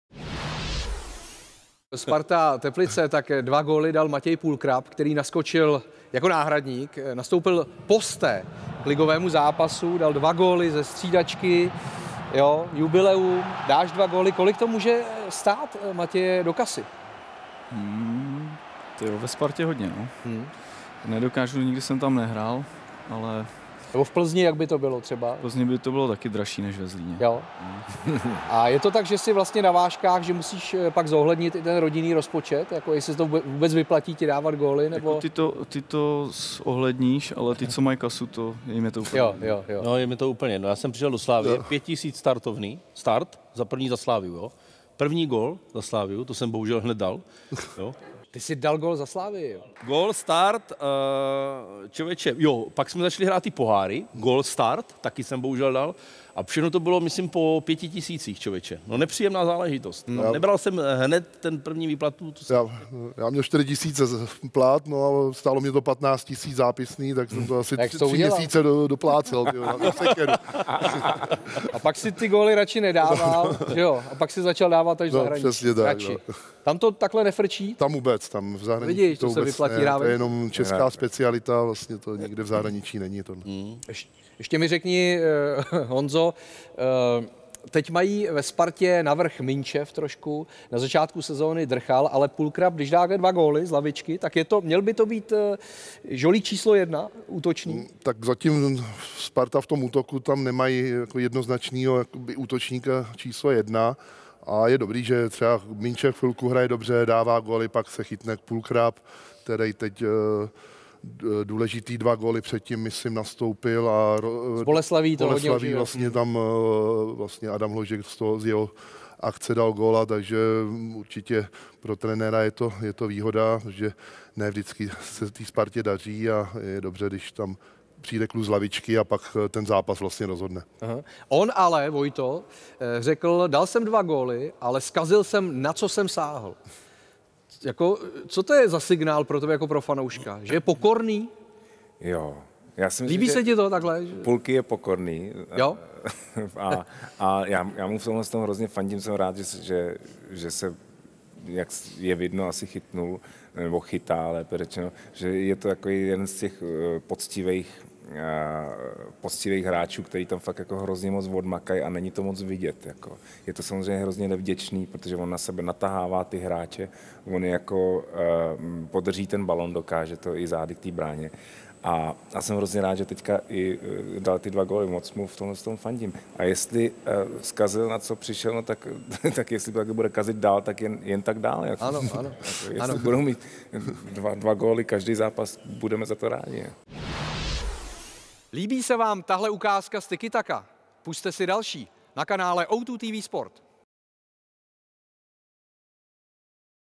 TIKI-TAKA: Pulkrab to bude mít drahé. A kolik vysolil ve Slavii Švancara?
Matěj Pulkrab dal jako náhradník dva góly Teplicím, navíc ve 100. ligovém zápase. Kolik ho to asi bude stát? A kolik platil po příchodu do Slavie do týmové kasy Petr Švancara?